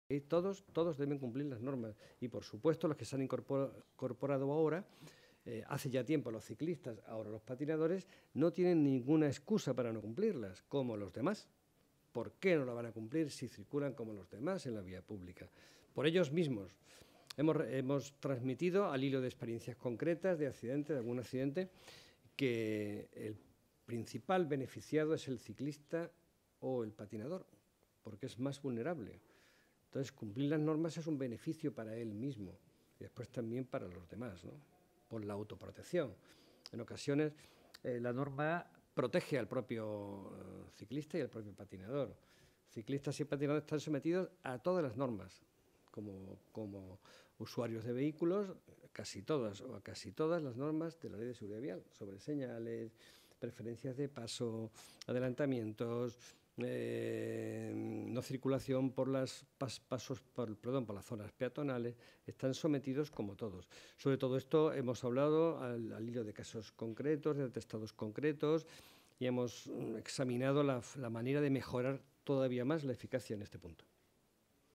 Fiscal de Tráfico: Ciclistas y patinetes también deben cumplir las normas
El fiscal de Sala del Tribunal Supremo, coordinador de Tráfico y Seguridad Vial, Bartolomé Vargas Cabrero, ha advertido de que ciclistas, patinadores y peatones también deben cumplir las normas de circulación, y ha resaltado la "necesidad" de que se apliquen estas normas a "todos" para conseguir una buena movilidad por las ciudades.